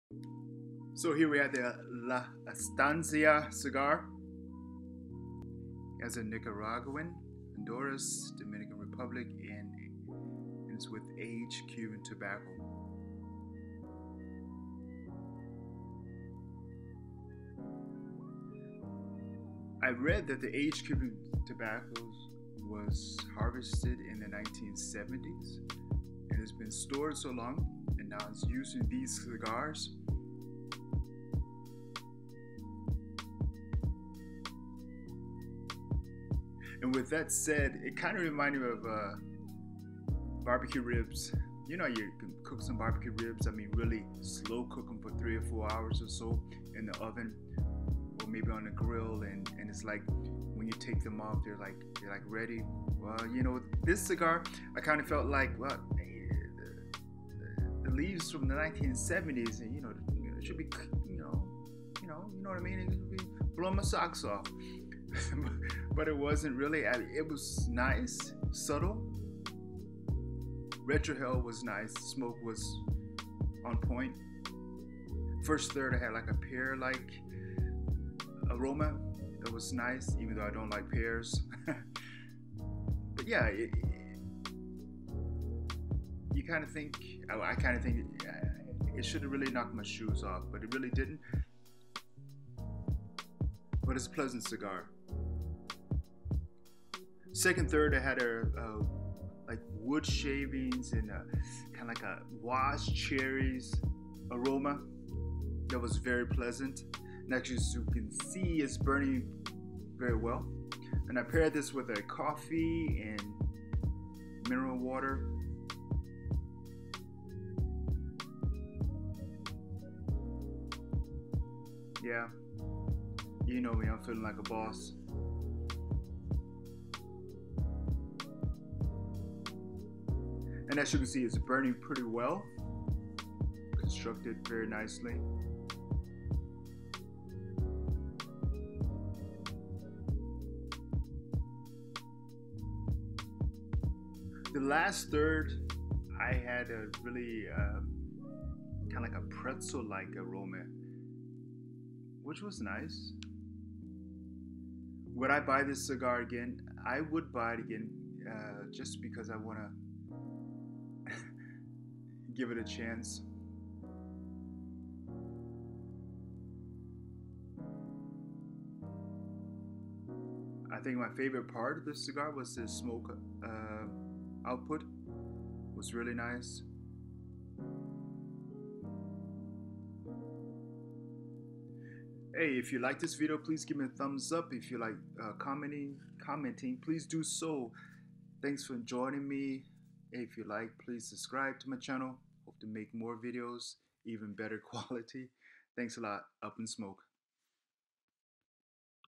La Estancia Cigar
A review of the La Estancia cigar. Nicaragua, Honduras, Dominican Republic, and aged cuban tobacco.